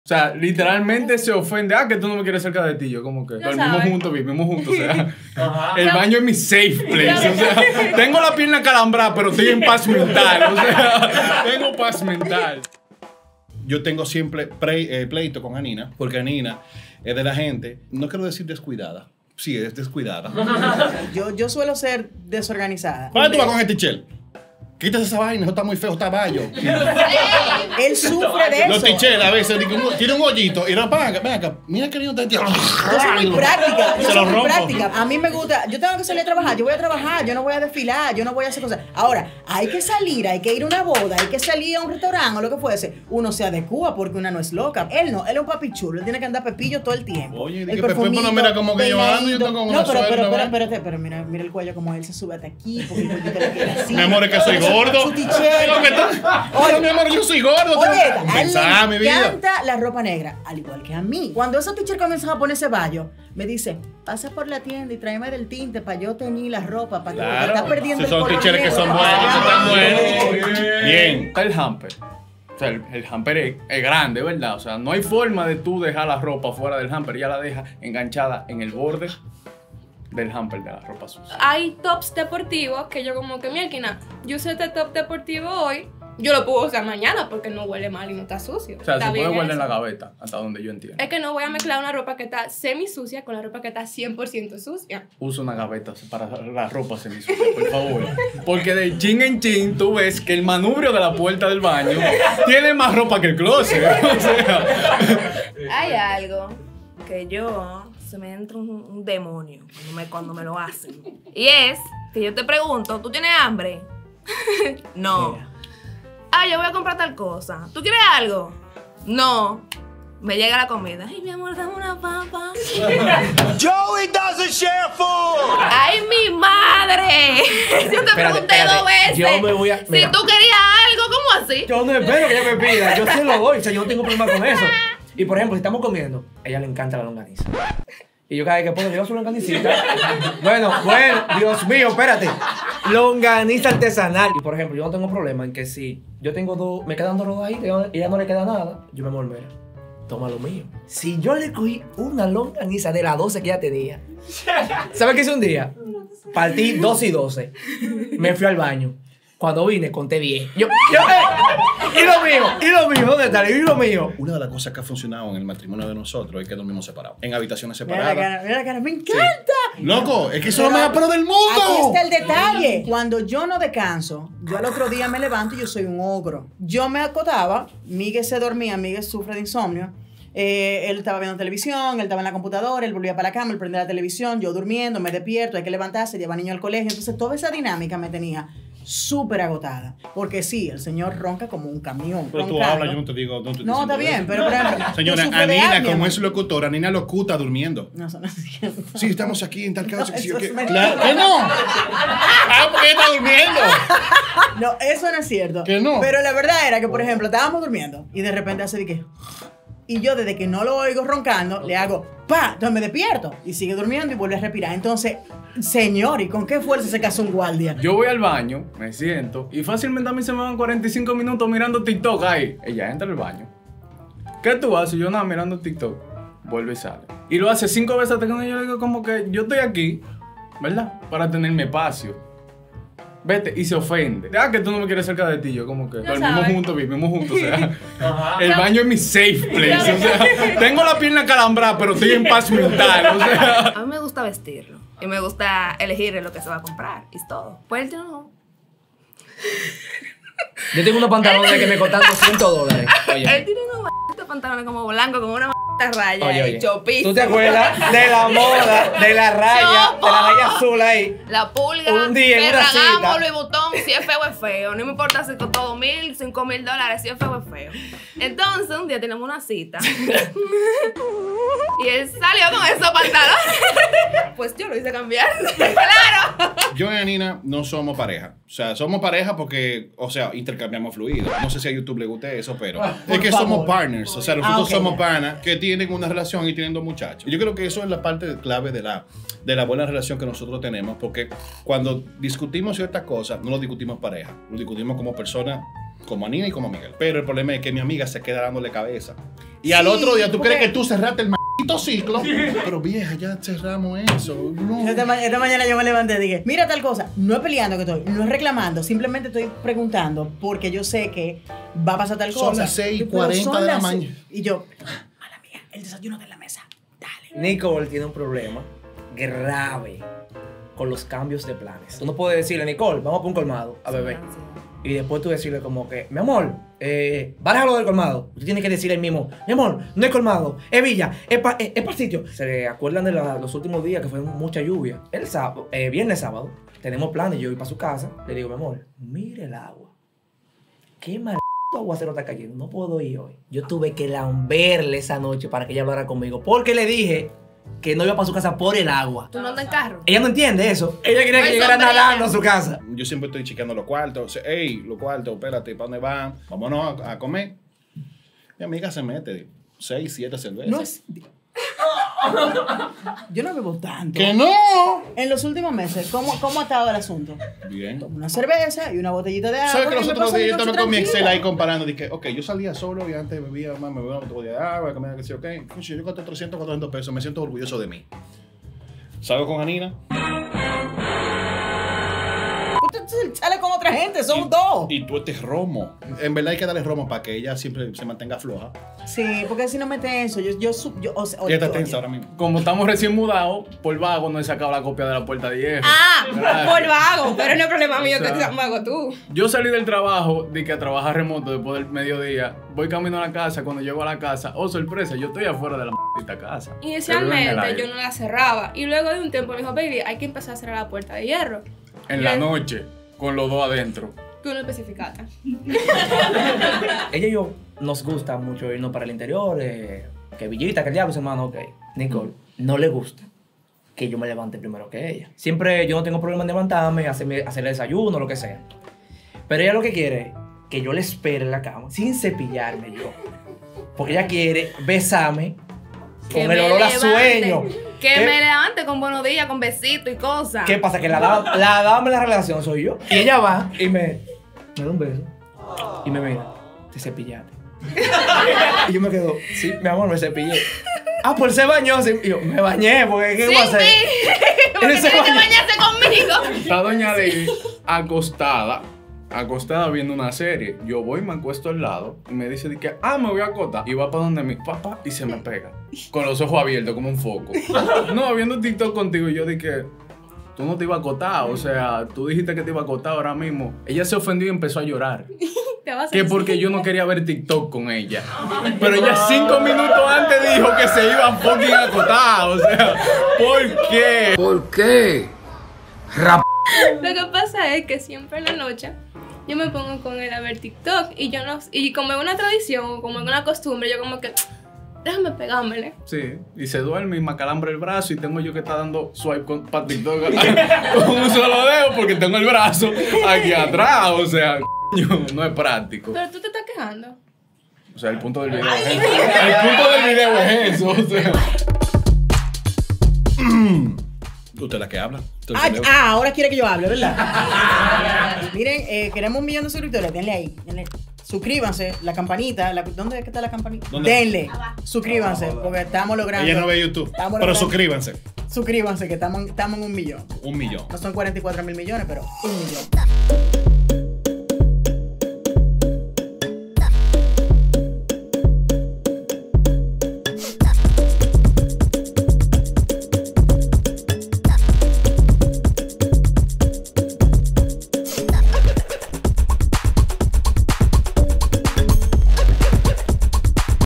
CONFESIONES entre PAREJAS - Tiradera entre LATINOS
En este video de TIRADERA entre PAREJAS, traemos novios latinos, quienes confiesan las discusiones y problemas mas comunes e incomodas de su relación!

Disfrutaras estas confesiones entre parejas !